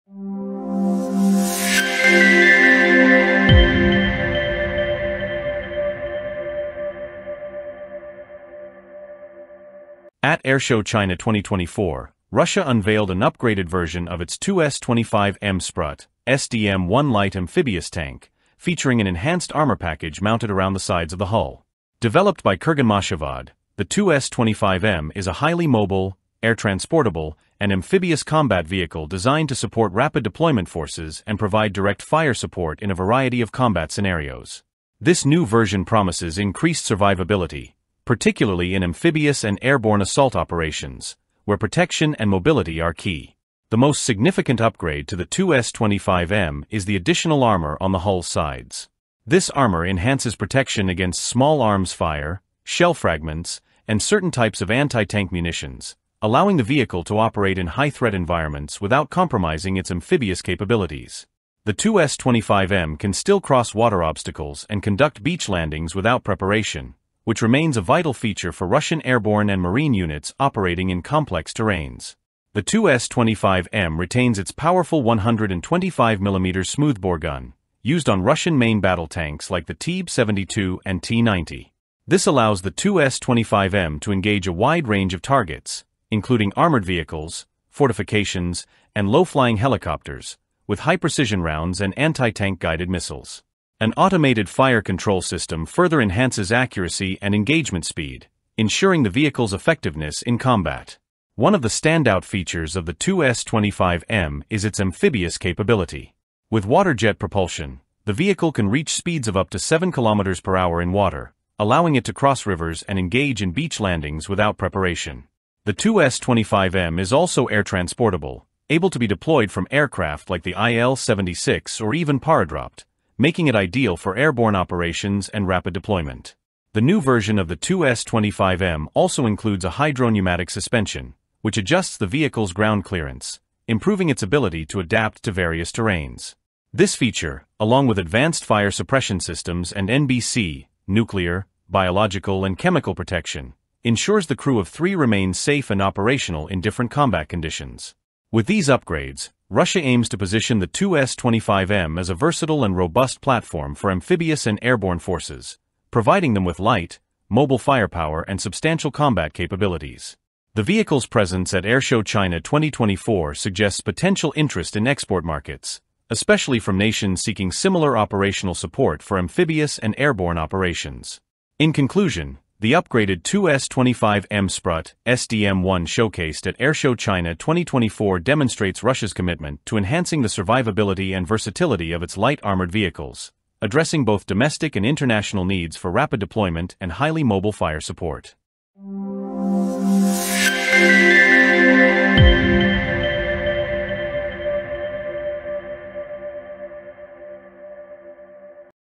Russian 2S25M Amphibious Tank with New Armor Package Debuts at AirShow China 2024
At AirShow China 2024, Russia revealed the upgraded 2S25M Sprut-SDM1, a light amphibious tank designed for rapid deployment and advanced combat scenarios. This new version features enhanced armor protection around the hull, increasing survivability against small arms fire, shell fragments, and anti-tank munitions. The 2S25M retains its powerful 125mm smoothbore gun and amphibious capabilities, allowing it to cross water obstacles and conduct beach landings. With improved mobility, firepower, and protection, the 2S25M is an ideal platform for amphibious and airborne operations. This upgrade positions the 2S25M as a versatile vehicle, capable of supporting modern military forces in high-threat environments and potentially appealing to international markets seeking similar capabilities.